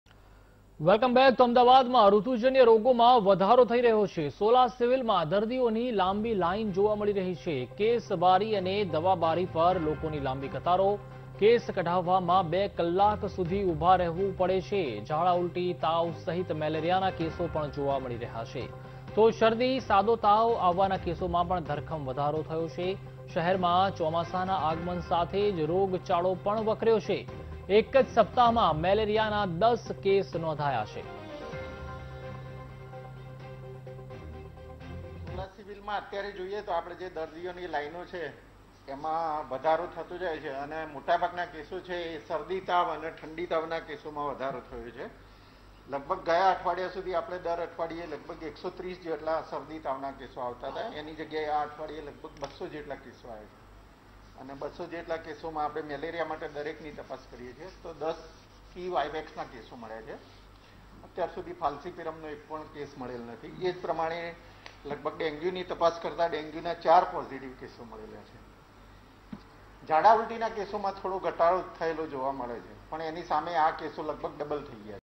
Ahmedabad માં ચોમાસાના આગમન સાથે જ વક્રયો રોગચાળો, શરદી-તાવ અને મલેરિયાના કેસોમાં ઘરખમ વધારો
Ahmedabad માં ચોમાસાના આગમન સાથે જ વક્રયો રોગચાળો, શરદી-તાવ અને મલેરિયાના કેસોમાં ઘરખમ વધારો | VTV GUJARATI
# AHMEDABAD